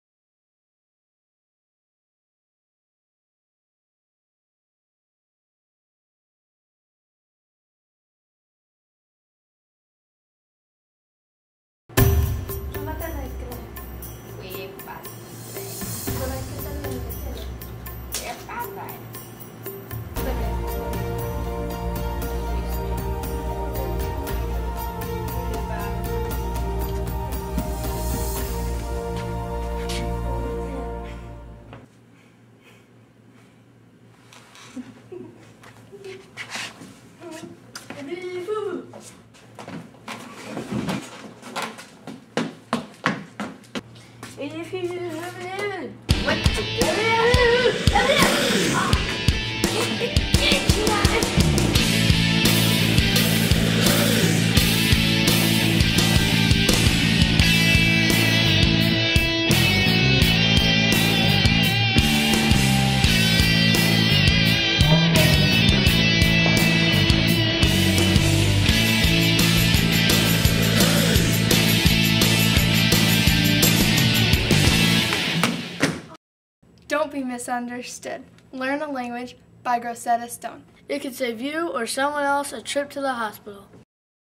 Grosetta Stone: student short film
A short mock commercial video written, directed, acted, and edited by junior high students as part of a 6 hour video workshop at the Helen Day Arts Center in Stowe, Vermont. Video class teacher: Adam Walker of Walker Run Productions. Students: Tavish, MacKenzie, Rowan.